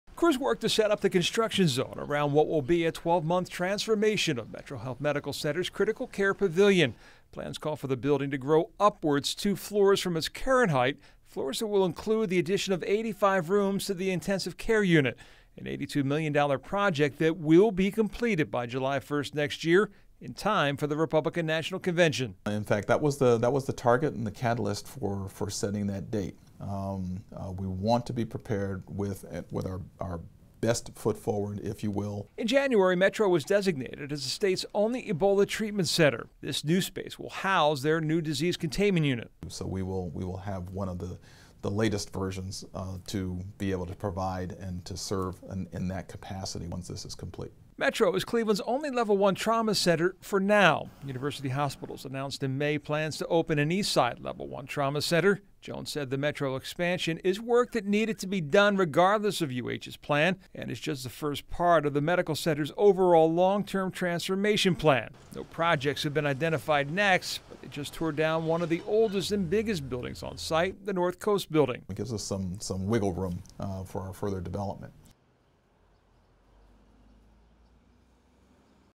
MetroHealth begins expansion of Critical Care Pavilion
Cleveland's MetroHealth Medical Center begins expansion of its Critical Care Pavilion